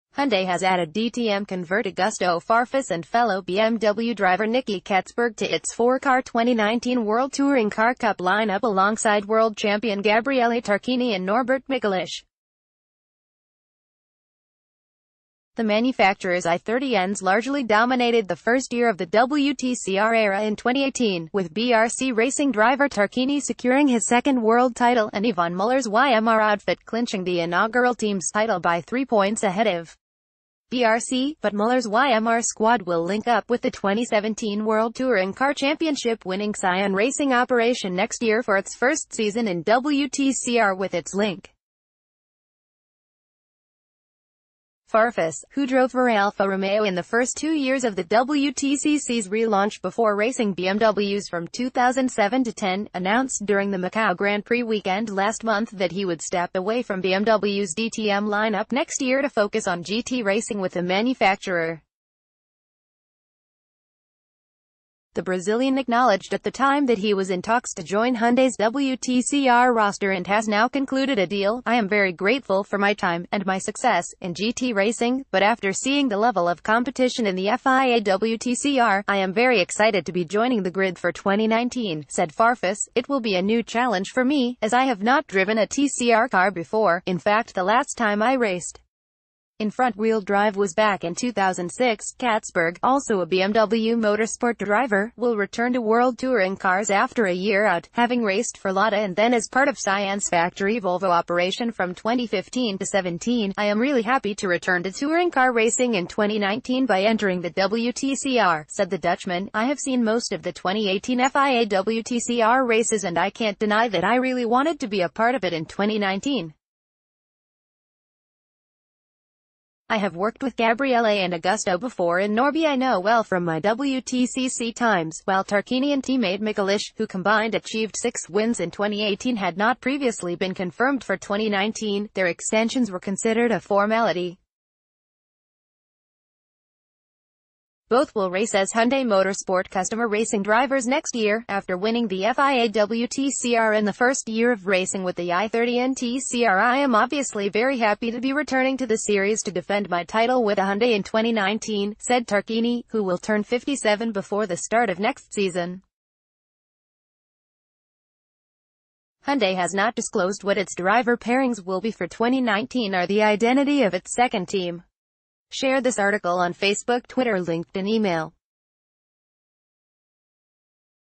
Farfus and Catsburg join Tarquini and Michelisz in Hyundai WTCR | CAR NEWS 2019
Hyundai has added DTM convert Augusto Farfus and fellow BMW driver Nicky Catsburg to its four-car 2019 World Touring Car Cup line-up alongside world c...